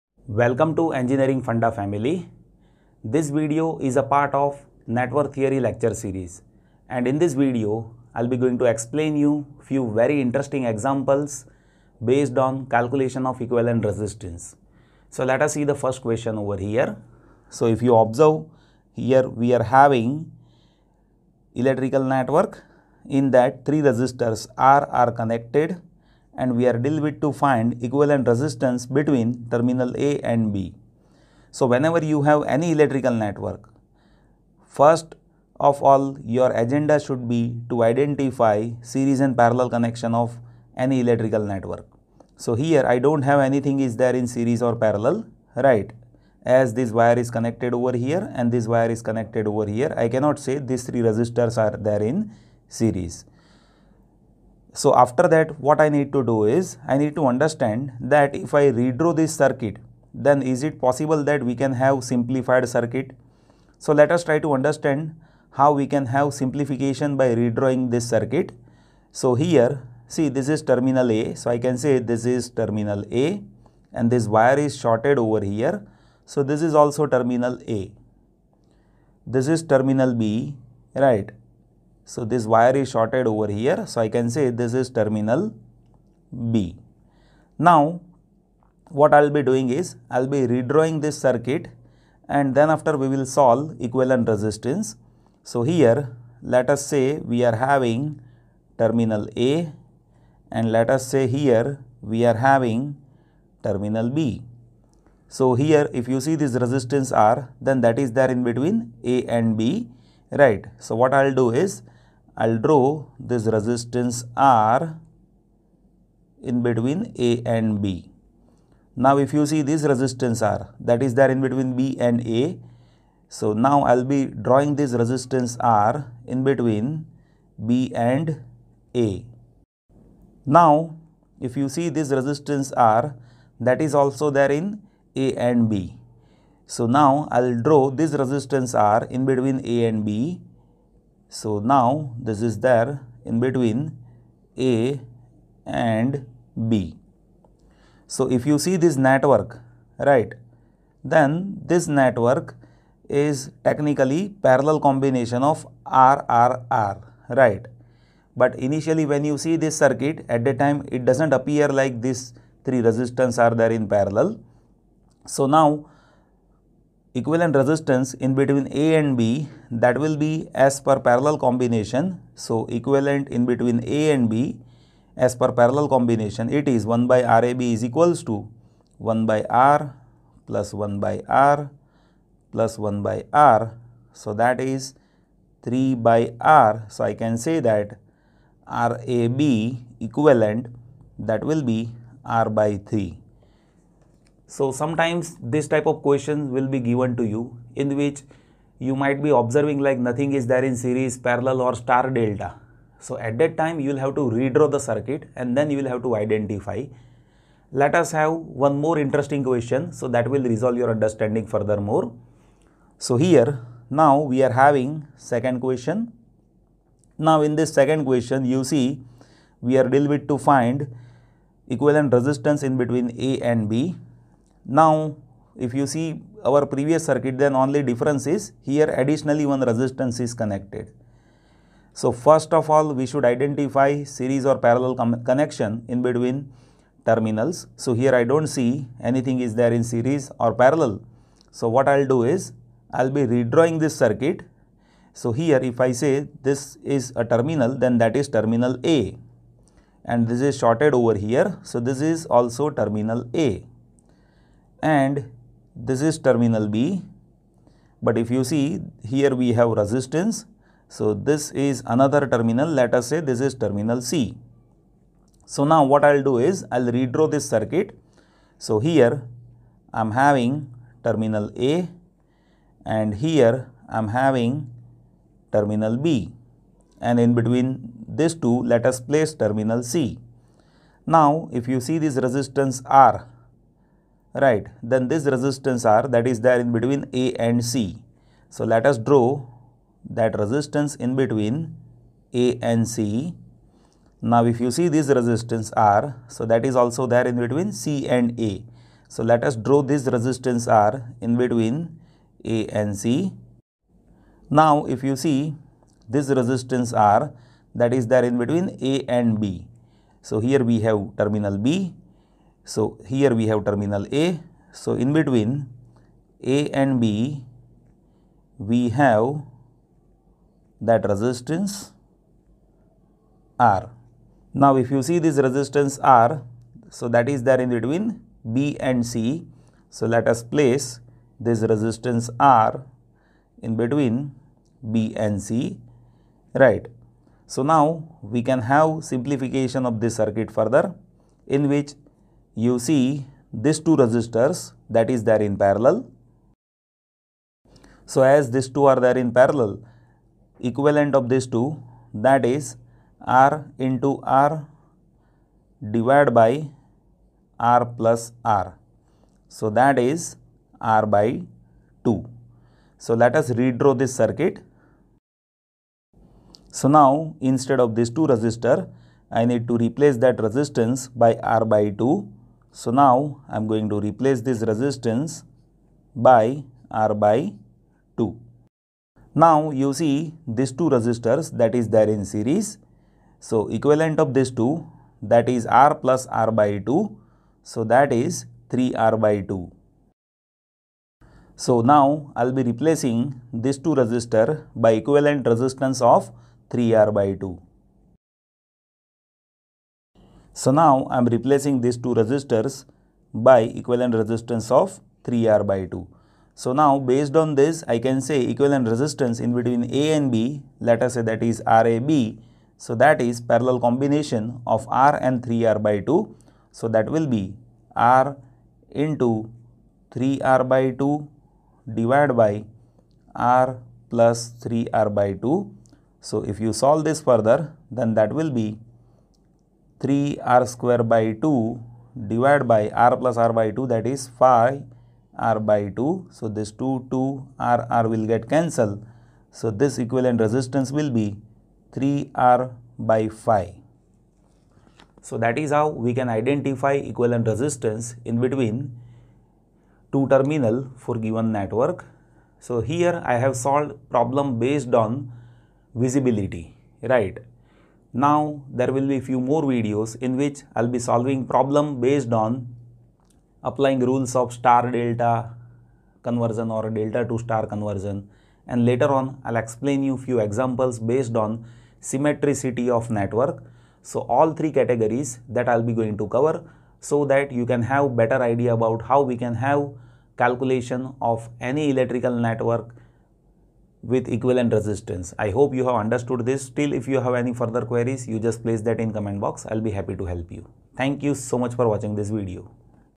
Equivalent Resistance Problems: Detailed Examples and Solutions 1
Equivalent Resistance Problems are covered by the following Timestamps:

0:00 - Network Theory Lecture Series - Equivalent Resistance Problems
0:17 - 1 Equivalent Resistance Problem
2:27 - Parallel Connection of resistance
3:12 - 1 Equivalent Resistance of given Network
3:27 - 2 Equivalent Resistance Problem
5:40 - Parallel Connection of resistance
6:19 - Series Connection of resistance
6:49 - 2 Equivalent Resistance of given Network

Equivalent Resistance Problem is covered by the following Outlines:

1. Network Theory Lecture Series
2. Equivalent Resistance Problem
3. Example on Equivalent Resistance
4. Equivalent Resistance of given Network
5. Series connection of resistance
6. Parallel Connection of resistance
7. Solved Problems of Equivalent Resistance

Engineering Funda channel is all about Engineering and Technology. Here this video is a part of Network Theory / Circuit and Network.